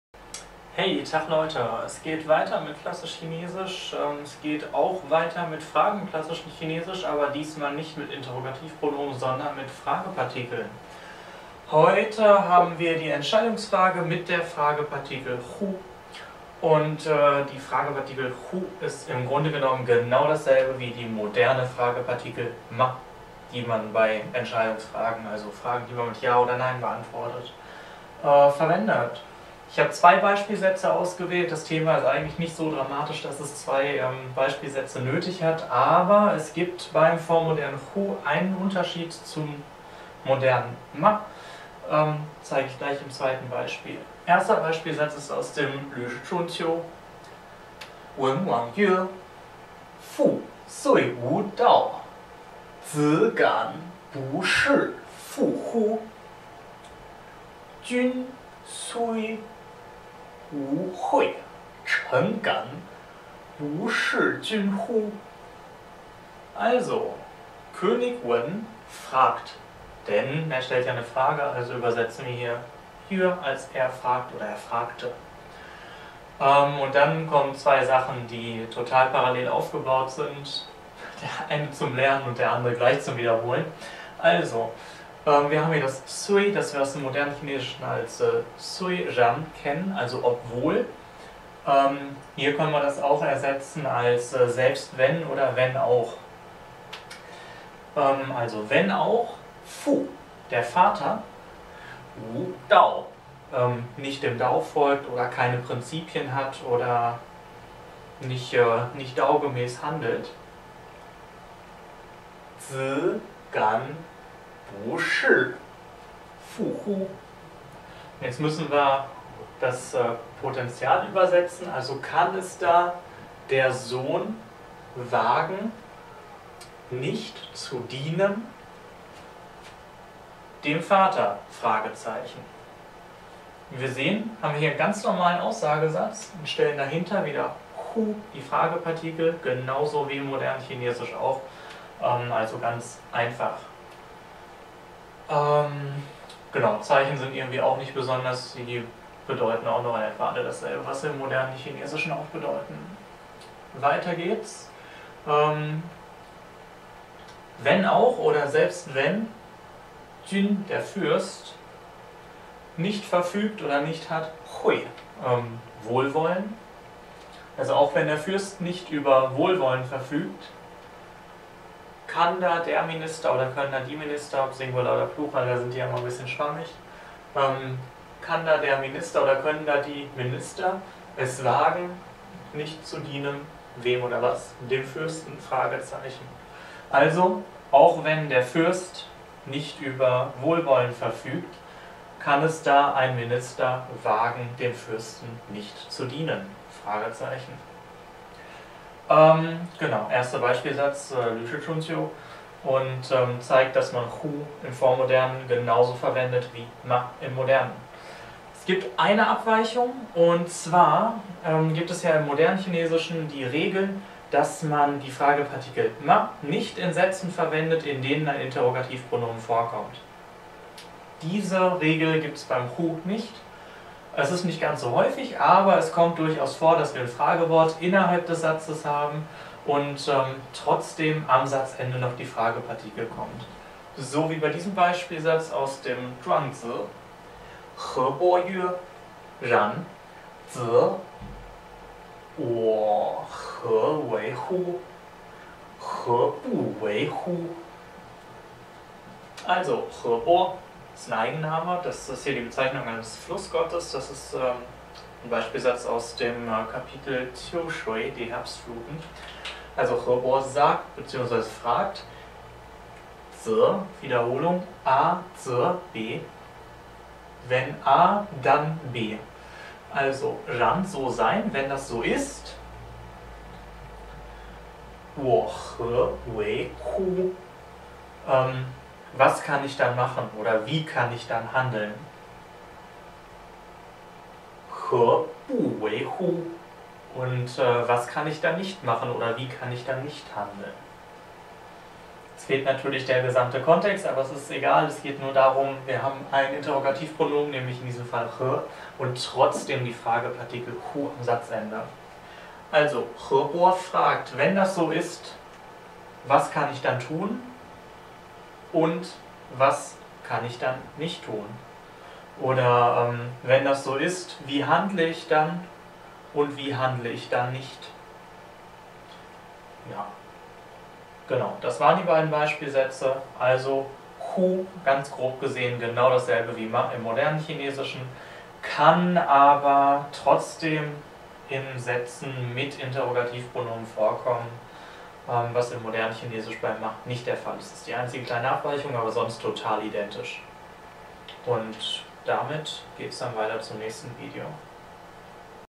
(43) Klassisches Chinesisch: Erweiterungsstufe 4-1 - Die Fragepartikel 乎
Dieses Video stellt die Fragepartikel 乎 vor. Sie wird für Entscheidungsfragen verwendet, wie die moderne Fragepartikel 嗎. Im Gegensatz zu dieser kann sie aber auch in Sätzen mit Interrogativpronomen stehen, was im modernen Chinesisch ausgeschlossen ist.

Dazu zwei Beispielsätze, einer aus dem Lüshi Chunqiu 呂氏春秋 und einer aus dem Zhuangzi 莊子.

Vokabeln:
文王 - wen2wang2: Eigenname; König Wen (Gründer der Zhou-Dynastie)
曰 - yue1: sagen; Einleitung der direkten Rede
父 - fu4: Vater
雖 - sui1: obwohl, obgleich, wenn auch, selbst wenn
無 - wu2: nicht geben, nicht haben, nicht existieren
道 - dao4: (moralische) Prinzipien; "dao-konformes Verhalten"
子 - zi3: Sohn, Kind
敢 - gan3: sich etw. wagen, sich trauen zu...
不 - bu4: Nein; nicht; un-
事 - shi4: Sache, (Staats-)angelegenheit, (Staats-)geschäfte; jdn. dienen, für jdn. arbeiten
乎 - hu1: Satzschlusspartikel für Entscheidungsfragen
君 - jun1: (Lehens-)fürst
惠 - hui4: Wohlwollen
臣 - chen2: Minister
河伯 - he2bo1: Eigenname; ein Flussgott; Hebo
然 - ran2: so sein
則 - ze2: wenn..., dann...
我 - wo3: ich; wir
何 - he2: was; wie; welche/r/s
為 - wei2: tun, machen, handeln; machen zu; ansehen als